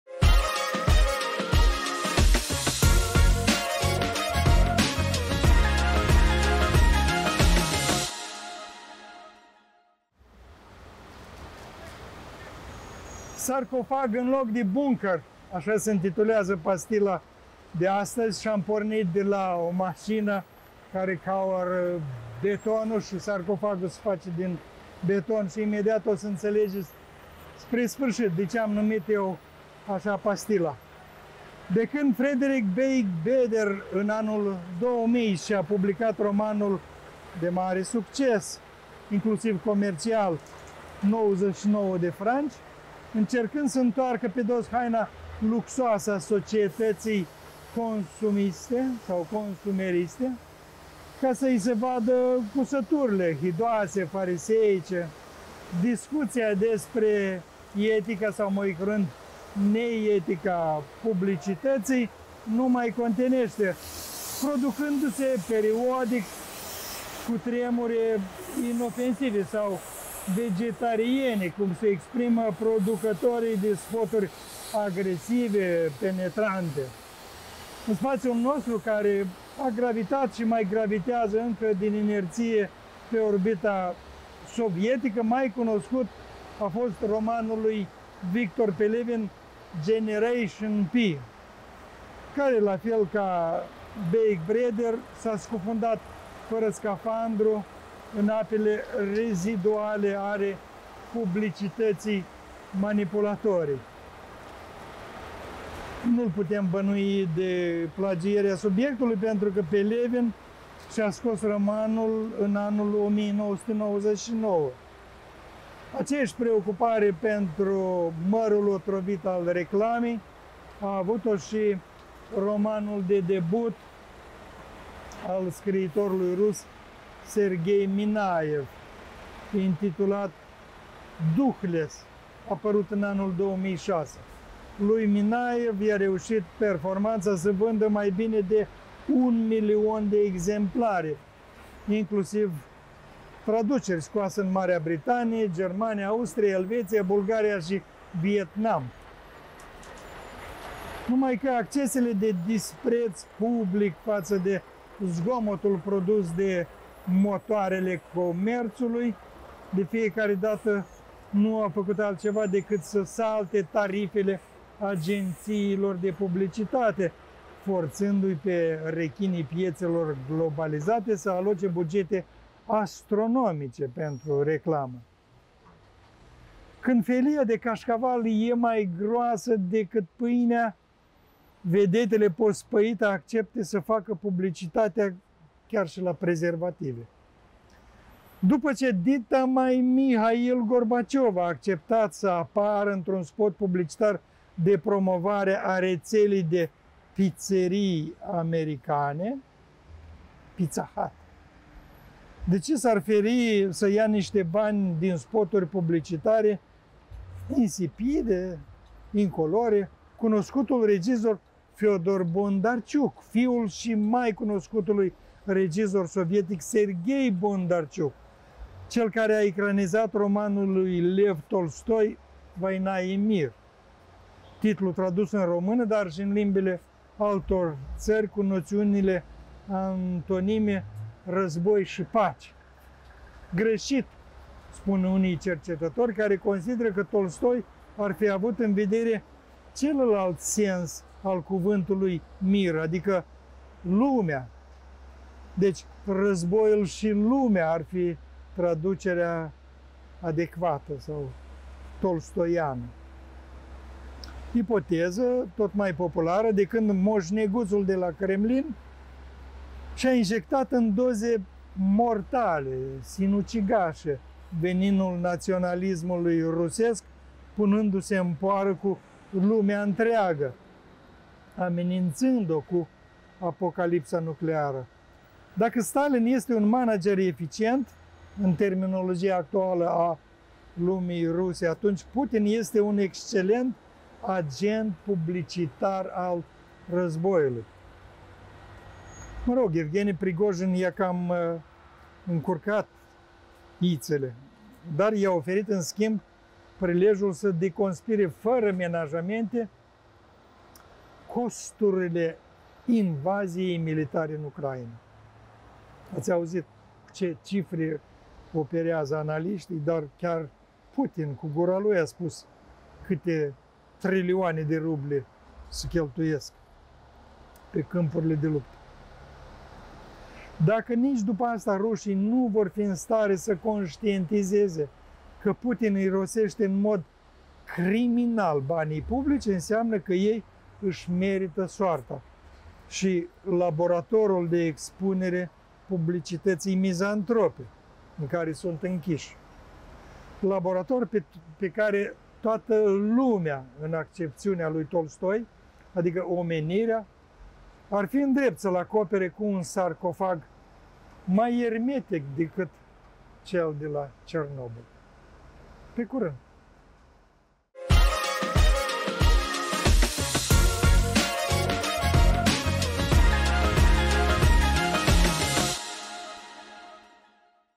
Minutul lui Vasile Botnaru- SARCOFAG ÎN LOC DE BUNCĂR!
De când Frederic Beigbeder în anul 2000 și-a publicat romanul de mare succes (inclusiv comercial!!!) „99 de franci” încercând să întoarcă pe dos haina luxoasă a societății consumeriste, ca să i se vadă cusăturile hidoase, fariseice, discuția despre (ne)etica publicității nu mai contenește producându-se  periodic mici cutremure inofensive sau vegetariene, cum se exprimă producătorii de spoturi agresive, penetrante.
În spațiul nostru care a gravitat și mai gravitează din inerție pe orbita sovietică mai cunoscut a fost romanul lui Viktor Pelevin „Generation P” care la fel ca Beigbeder s-a scufundat fără scafandru în apele reziduale ale publicității manipulatorii.  Nu-l putem bănui de plagierea subiectului pentru că Pelevin și-a scos romanul în anul 1999. Aceeași preocupare pentru mărul otrăvit al reclamei a avut-o și romanul de debut al scriitorului rus Serghei Minaiev, intitulat „DUHLESS”  apărut în anul 2006. Lui Minaiev i-a reușit performanța să vândă mai bine de un milion de exemplare, inclusiv traduceri scoase în Marea Britanie, Germania, Austria, Elveția, Bulgaria și Vietnam..
Numai că accesele de dispreț public față de zgomotul produs de motoarele comerțului de fiecare dată nu au făcut altceva decât să salte tarifele agențiilor de publicitate forțându-i pe rechinii piețelor globalizate să aloce bugete astronomice pentru reclamă.  Când felia de cașcaval e mai groasă decât pâinea, vedetele pospăite acceptă să facă publicitatea chiar și la prezervative.
După ce ditamai Mihail Gorbaciov a acceptat să apară într-un spot publicitar de promovare a rețelei de pizzerii americane,  de ce s-ar feri să ia niște bani din spoturi publicitare insipide cunoscutul regizor Fiodor Bondarciuk, fiul și mai cunoscutului regizor sovietic Serghei Bondarciuk, cel care a ecranizat romanul lui Lev Tolstoi „Voina i Mir”, titlu tradus în română dar și în limbile altor țări cu noțiunile antonime RĂZBOI ȘI PACE. Greșit! spun unii cercetători care consideră că Tolstoi ar fi avut în vedere celălalt sens al cuvântului MIR, adică LUMEA, deci „Războiul și lumea” ar fi traducerea adecvată. Ipoteză tot mai populară de când moșneguțul de la Kremlin și-a injectat în doze mortale/sinucigașe veninul naționalismului rusesc punându-se în poară cu LUMEA întreagă,  amenințând-o cu apocalipsa nucleară. Dacă Stalin este un „manager eficient”, în terminologia actuală a lumii ruse, atunci Putin este un excelent agent publicitar al războiului. Evgheni Prigojin i-a cam încurcat ițele, dar i-a oferit în schimb prilejul să deconspire fără menajamente costurile invaziei militare în Ucraina. Dacă nici după asta rușii nu vor fi în stare să conștientizeze că Putin irosește în mod criminal banii publici, înseamnă că ei își merită soarta și laboratorul de expunere publicități mizantrope în care sunt închiși, laborator pe care lumea, în accepțiunea lui Tolstoi, adică omenirea ar fi în drept să-l acopere cu un sarcofag mai ermetic decât cel de la Cernobîl.